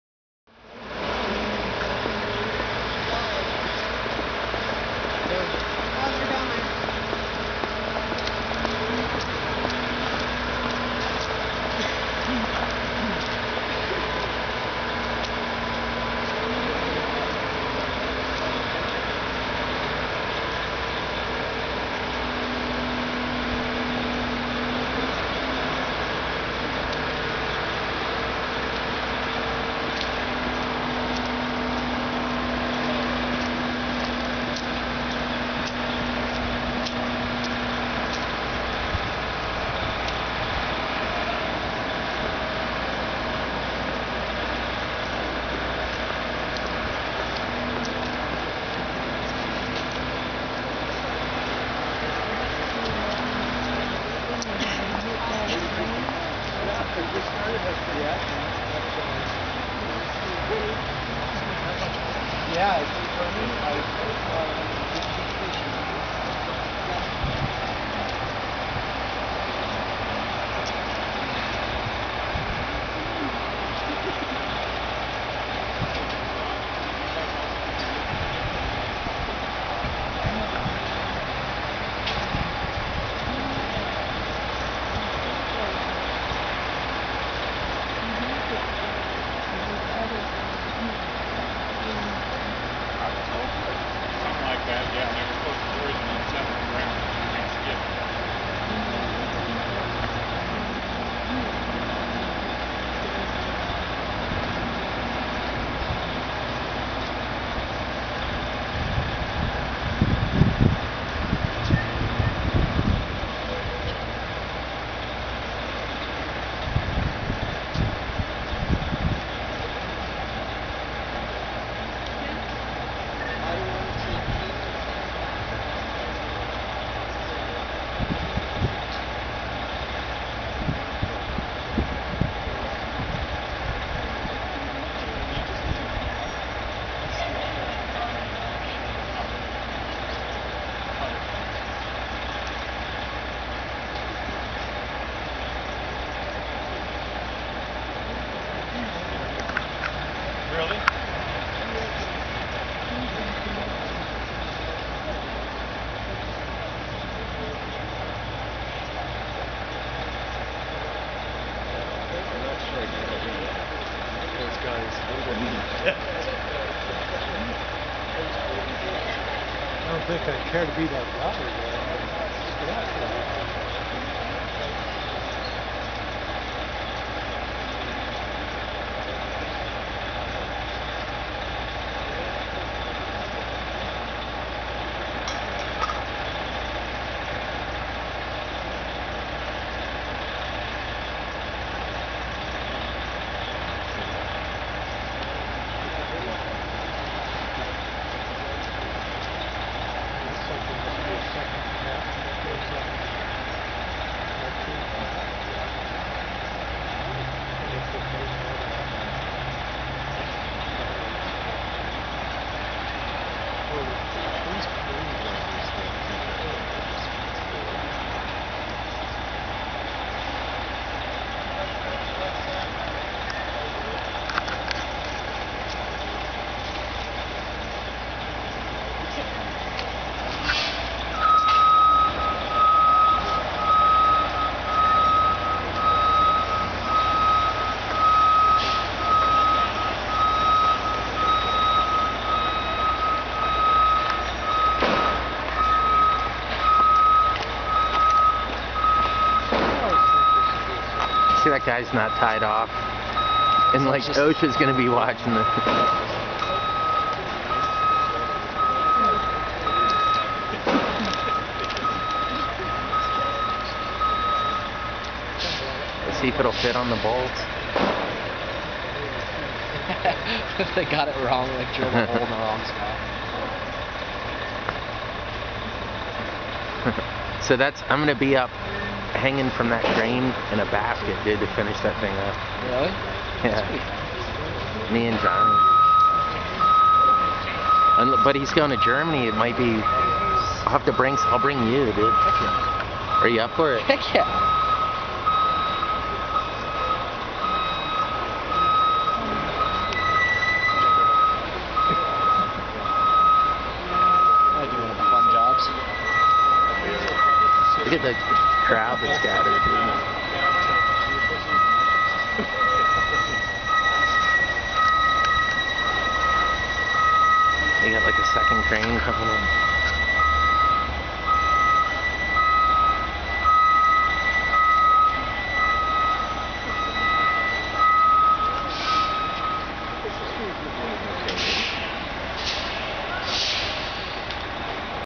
Nagel tower
The spire at Nagel Hall being lifted on to the building. Mike did all the wood construction on this spire.